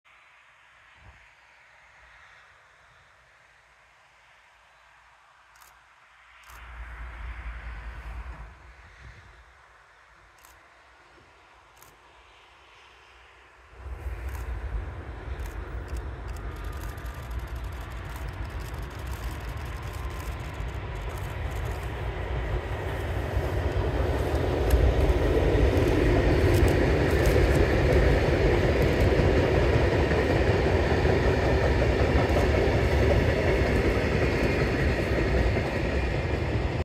LEG 132 158 bei Sonnenaufgang mit Kalkkübelzug in Blankenburg (Harz)
Fotogüterzug mit LEG 132 158 und 10 Kalkkübel von Blankenburg kommend mit Blick auf die A36 Brücke.